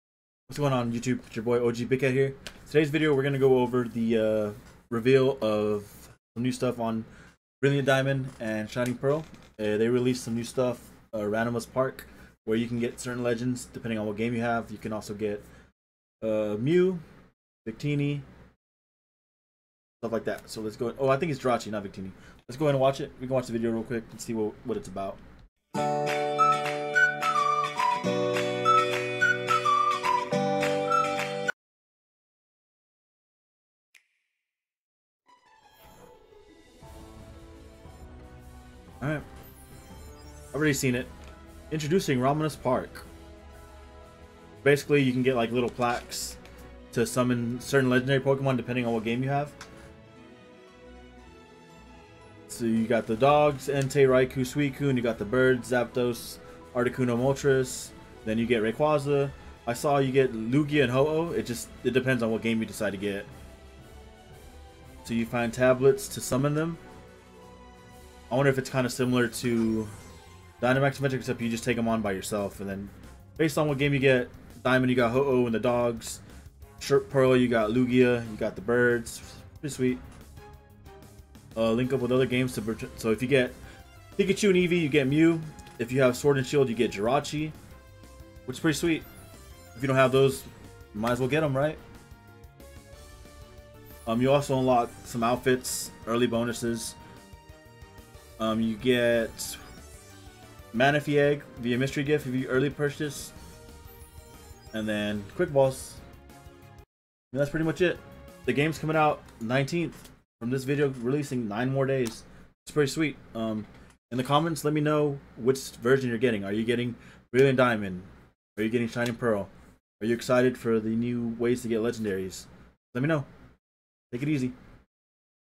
Pokemon Brilliant Diamond and Shining Pearl Reveals! Ramanas Park? Free Mew and Jirachi?
In this video we go over the new info for Brilliant Diamond and Shining Pearl!